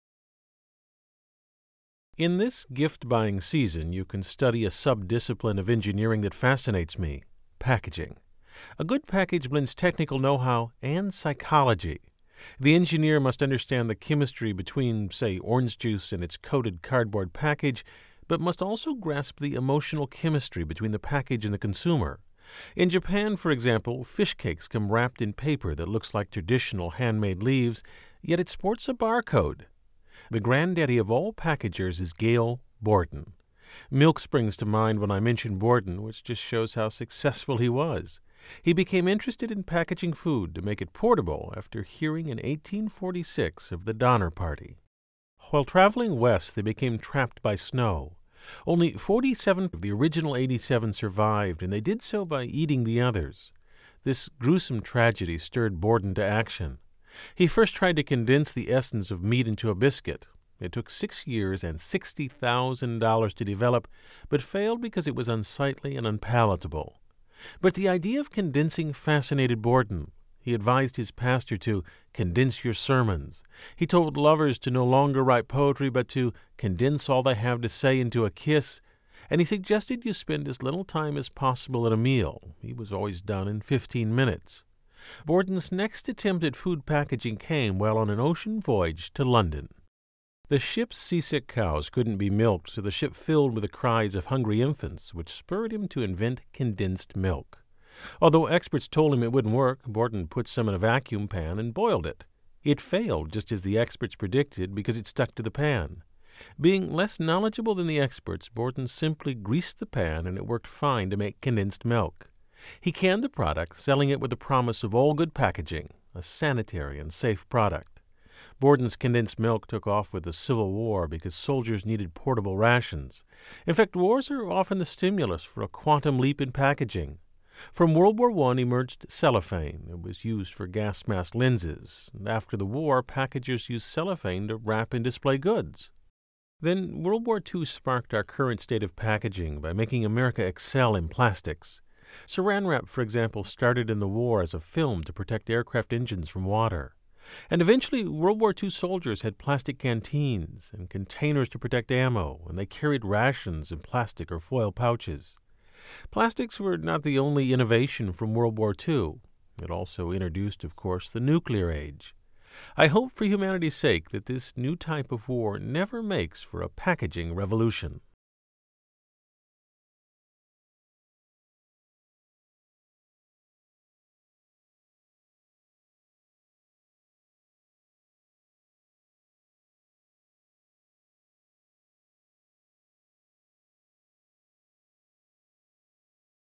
How the Donner Party inspired food packaging
Bill tells us about packaging, a sub-discipline of engineering that is essential to our society. This radio commentary was originally broadcast on November 30, 2004. Visit this link to view complete list of media attributions